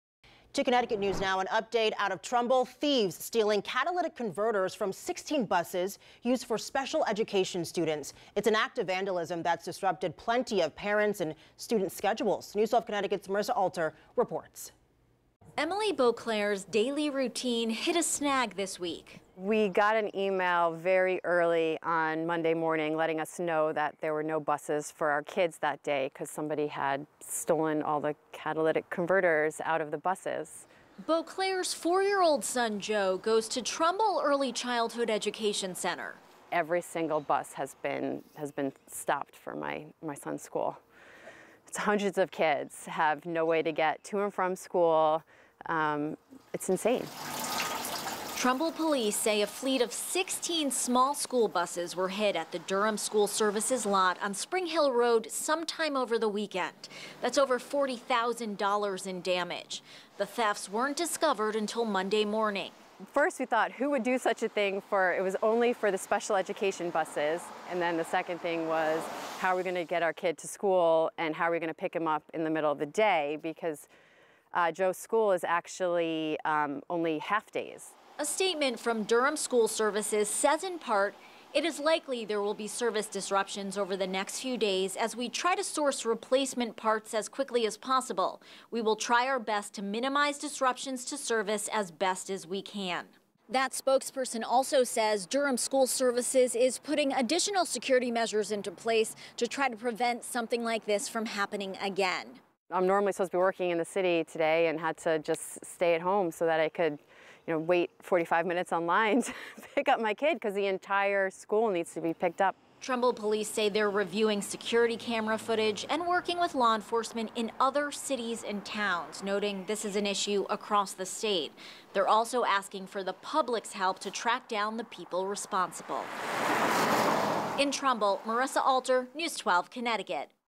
Bus service disrupted for Trumbull special education students after catalytic converter thefts
Trumbull police say a fleet of 16 small school buses were hit at the Durham School Services lot on Spring Hill Road sometime over the weekend, totaling a loss of over $40,000 dollars.